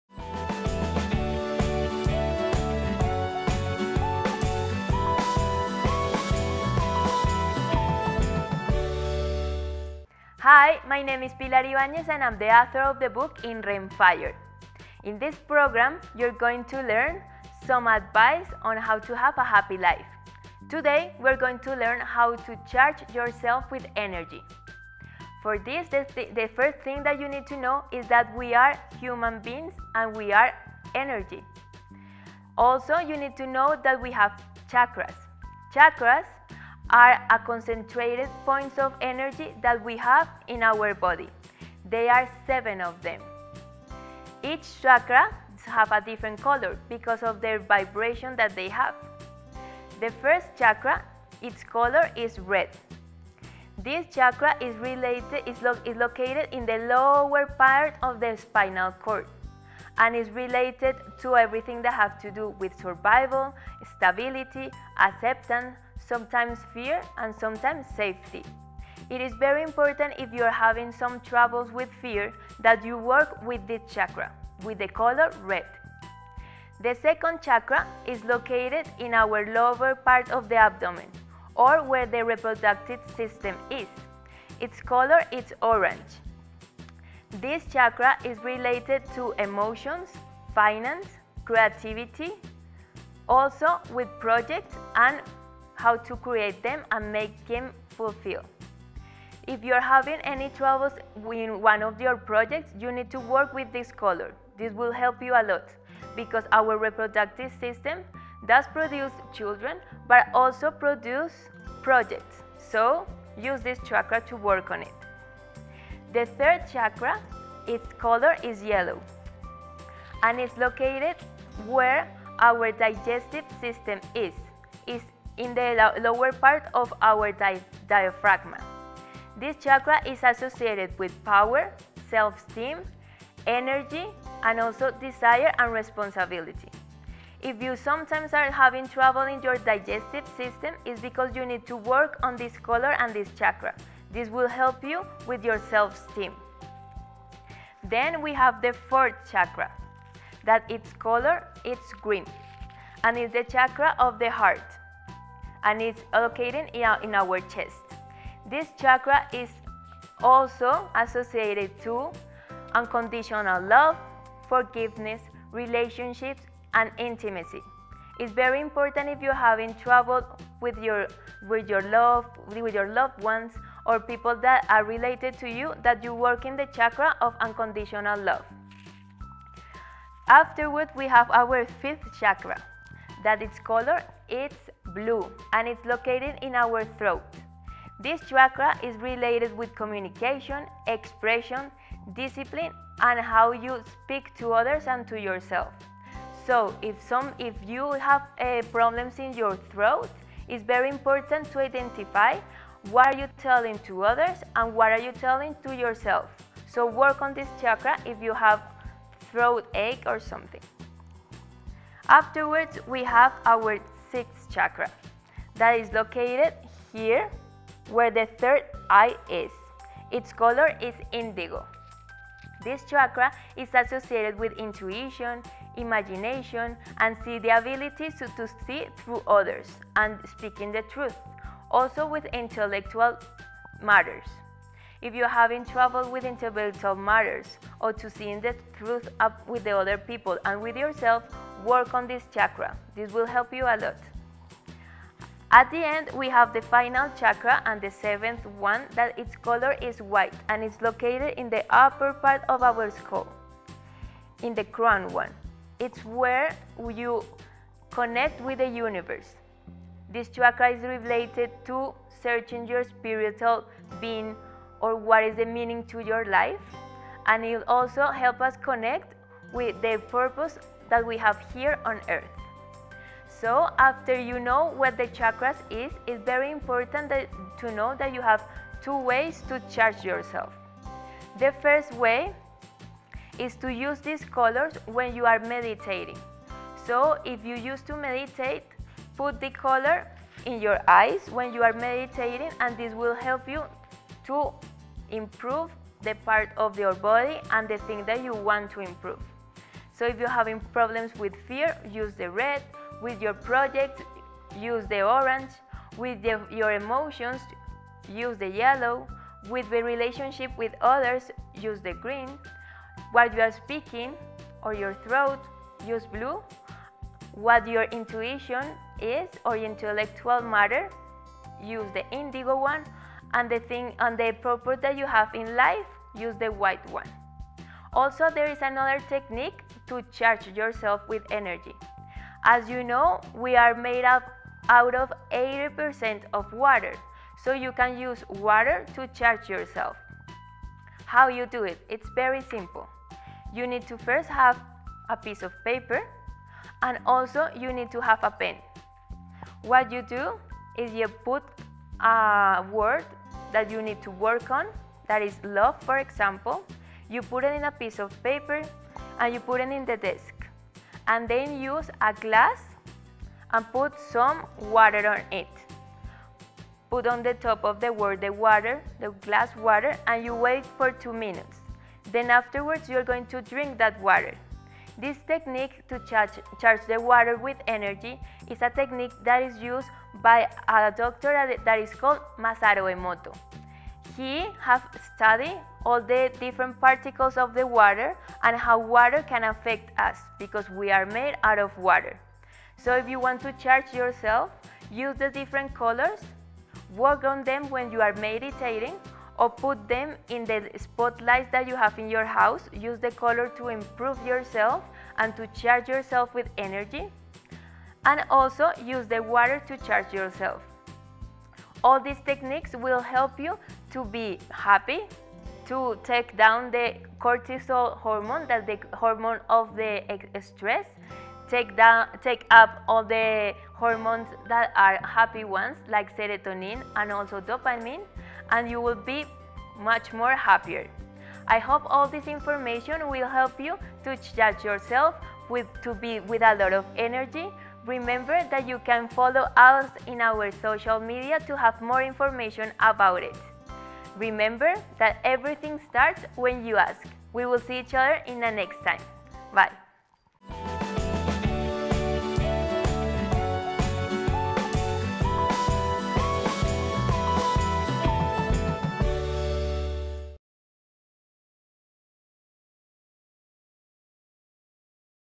Como Cargarte con Energias (ingles)- Hogar Tv por Juan Gonzalo Angel
La palabra chacra proviene del sáncrito. Lengua y filosofía hindú, y de sus textos sagrados , los vedas. Significa Disco o Rueda . Hace referencia a los distintos centros energéticos que tenemos distribuidos por el cuerpo, especialmente a los 7 chacras principales.

Siete son los colores del arco iris , siete notas musicales o los siete días de las semana, ya que son siete es el numero completo.

La energía en estos puntos sebe circular siempre de forma libre , ya que si se bloquea puede causar síntomas de enfermedad o malestar en nuestro organismo. La energía en estos puntos debe circular siempre de forma libre , ya que son las causas de que dicha desintonización en ellos provoque la enfermedad.